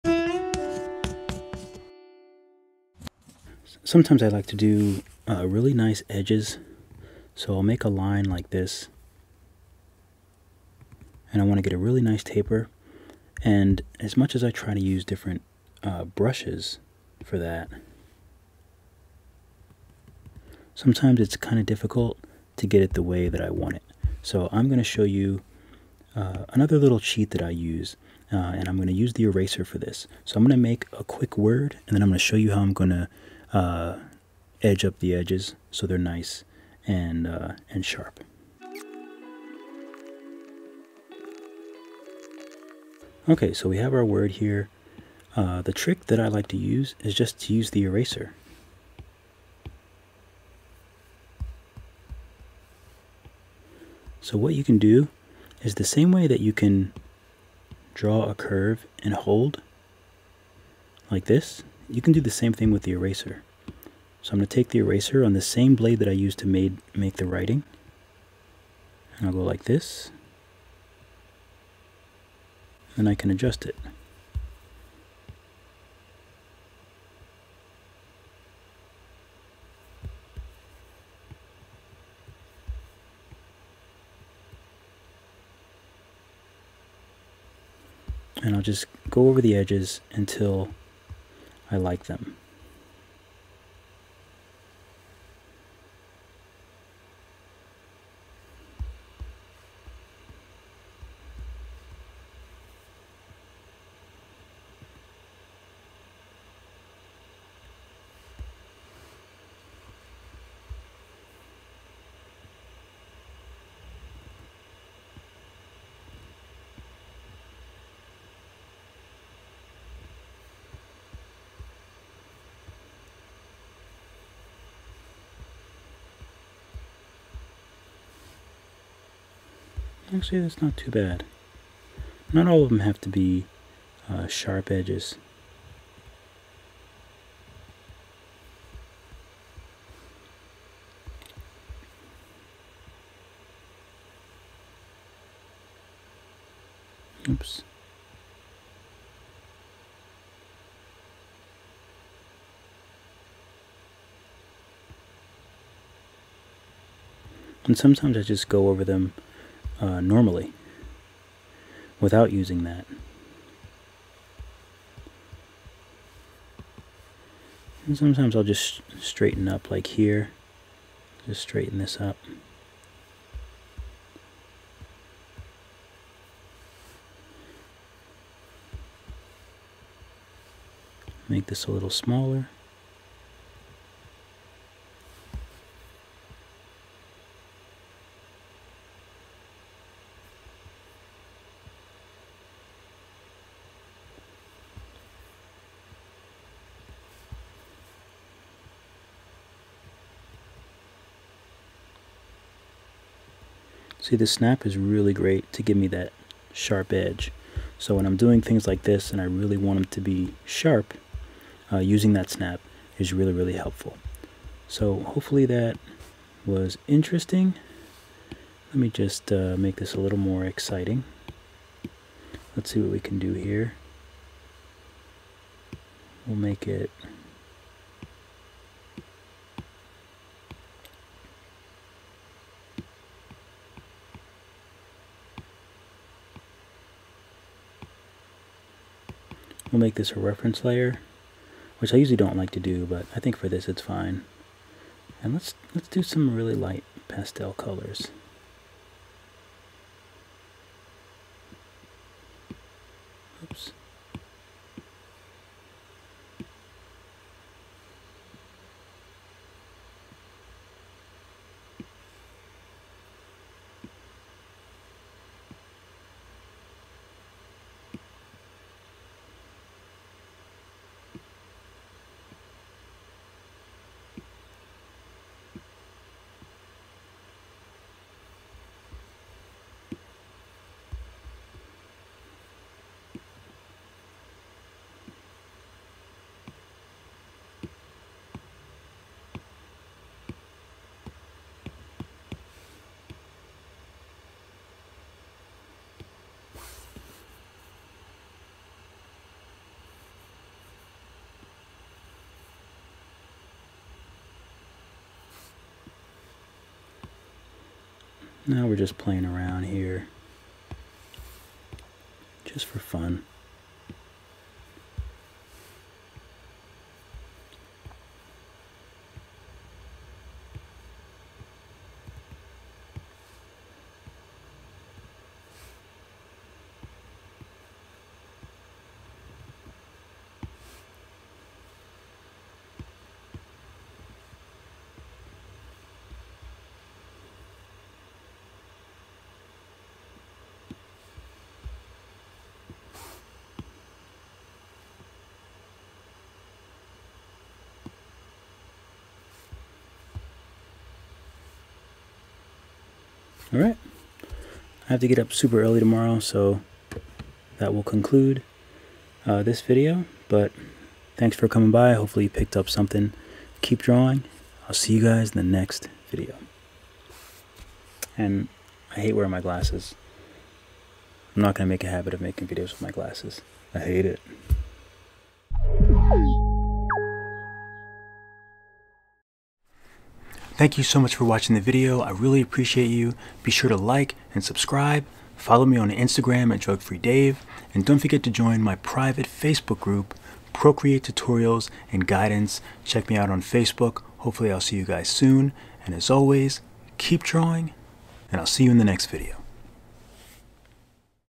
How to make CLEAN edges in Procreate using the Eraser - Tutorial
The eraser is essentially just an invisible brush. With the snapping feature, you can use it to make the crispiest edges you could need. I'm using BUTTERBLADE brush, you can download all of my brushes for free below.

--- DRAWING & TUTORIAL EQUIPMENT
(Screen Recorded with Iphone 6)

--- AUDIO ---
- Rode Video Micro Compact Shotgun Mic